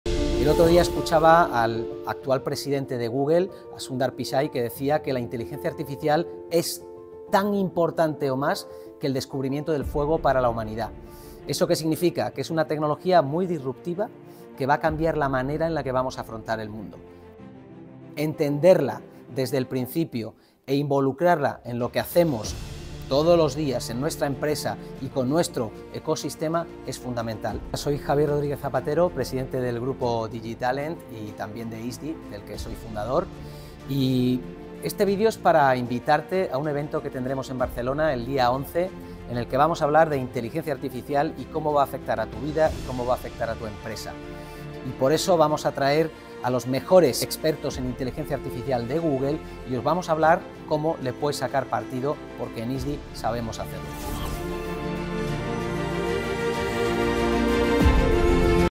Evento: AI & Leadership, setting the vision
Google e ISDI se unen en este especial evento para reflexionar sobre la IA y sobre cómo equipar a perfiles de responsabilidad con el conocimiento necesario para tomar decisiones estratégicas y, así, promover la adopción de la Inteligencia Artificial con claro impacto en el negocio a largo plazo, evitando los errores más habituales.

Esta sesión ofrece un foco claro en negocio y en cómo hacer uso de la IA para remodelar el modelo de negocio actual y generar oportunidades de crecimiento. El enfoque ayuda al participante a diseñar propuestas de valor nuevas y diferenciales con clara aplicación práctica, entendiendo su impacto directo en el liderazgo y la gestión empresarial. Desde la optimización de operaciones hasta la redefinición de productos y servicios, los asistentes saldrán con una visión clara de cómo aplicar la IA para alcanzar el éxito empresarial.